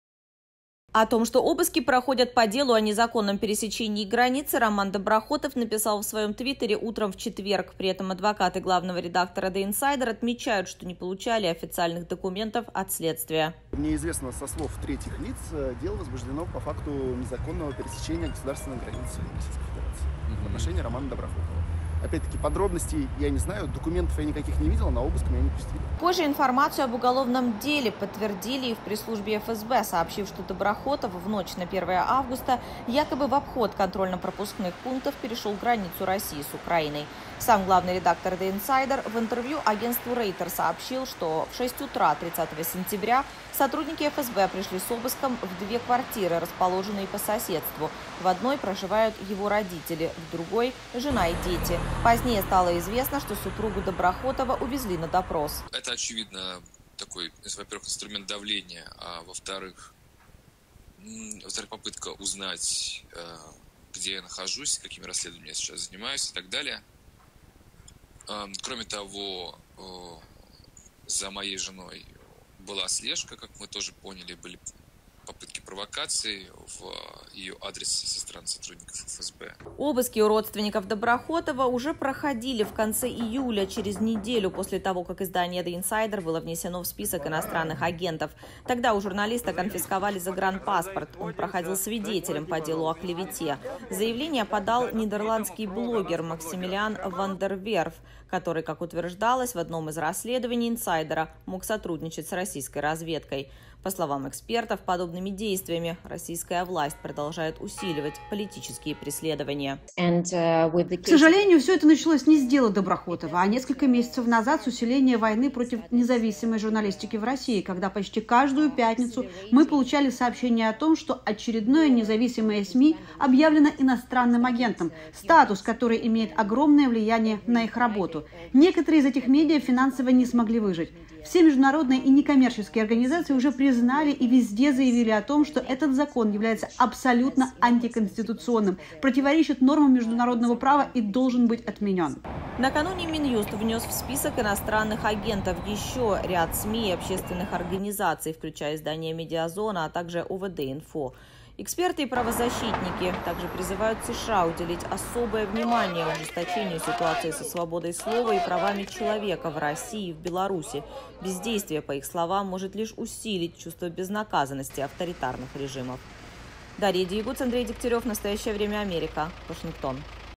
Главный редактор издания The Insider объявлен в розыск
Романа Доброхотова обвиняют в незаконном пересечении границы